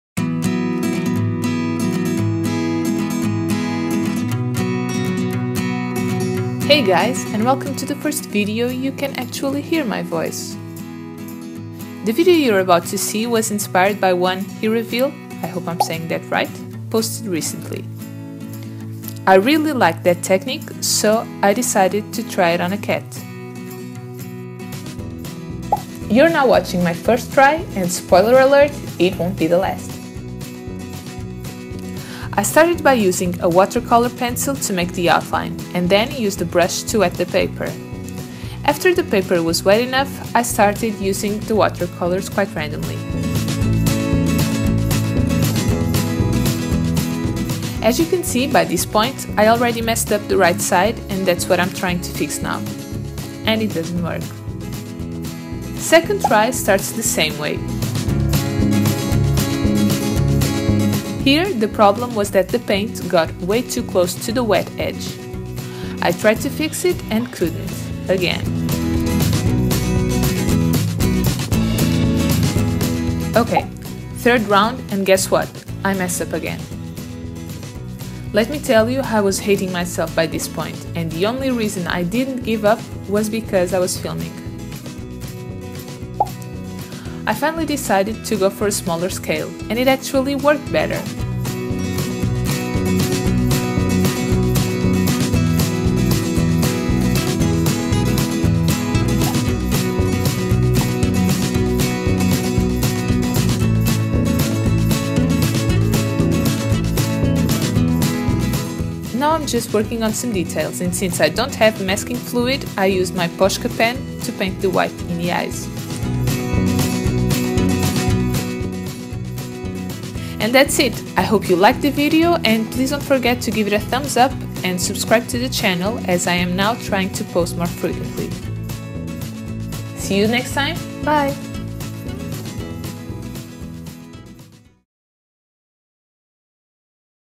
Drawing a Watercolor Fuzzy Cat - Trial and Error process video
In this video I show you the process of trying (and failing) to draw a watercolor cat using the wet on wet technique. I hope you enjoy it.

MY LINKS